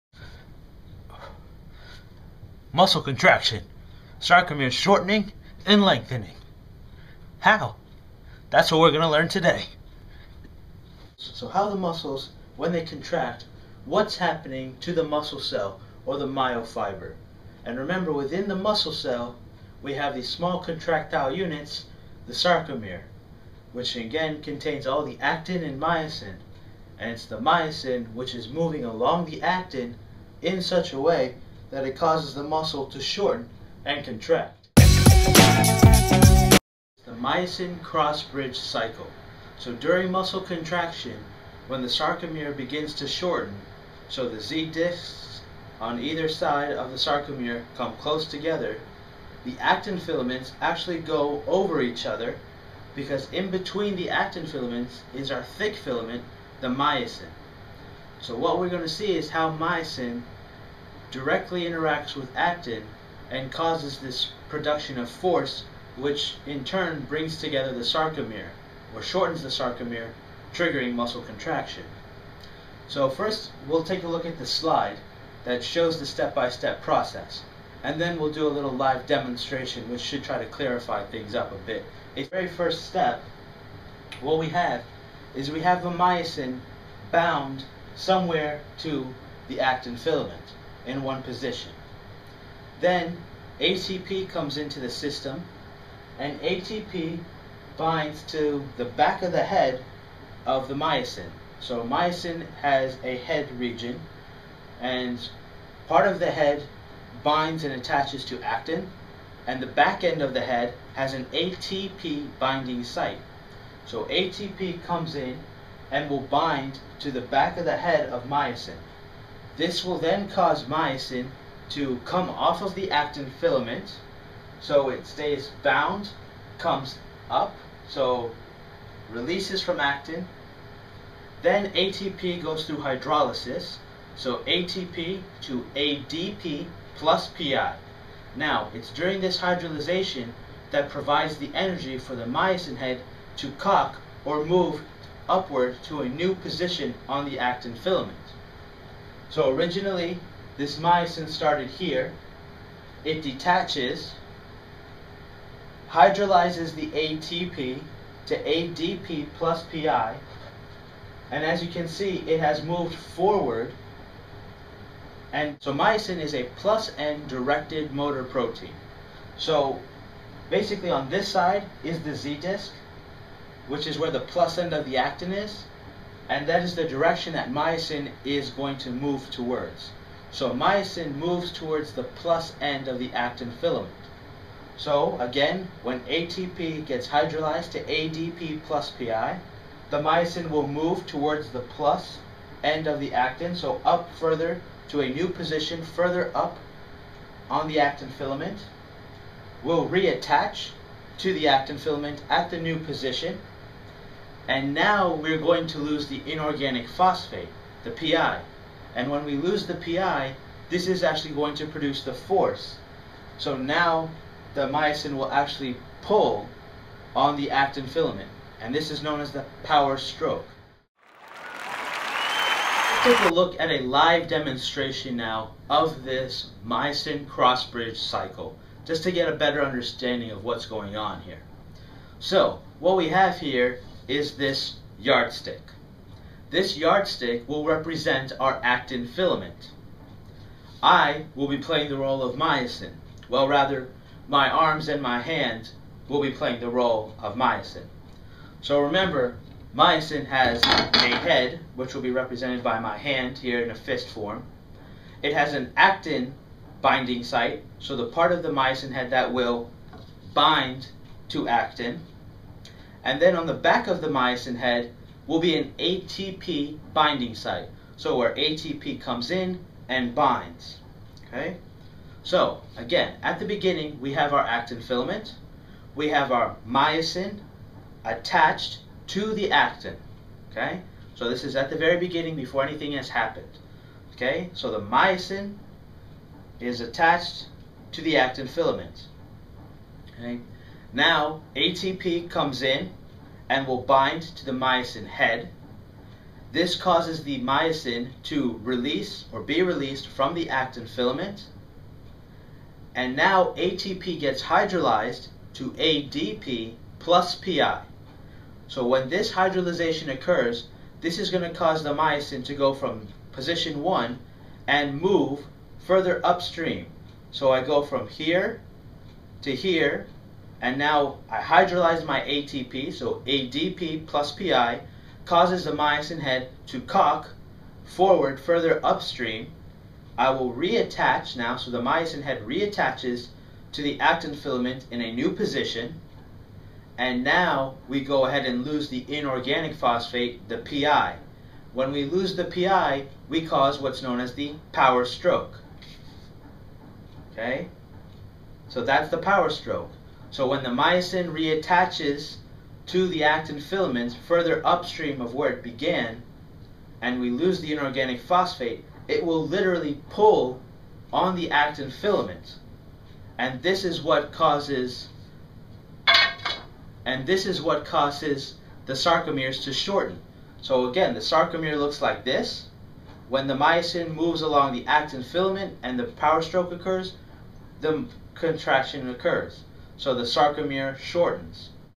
Myosin Cross-Bridge Cycle
How myosin produces force and interacts with actin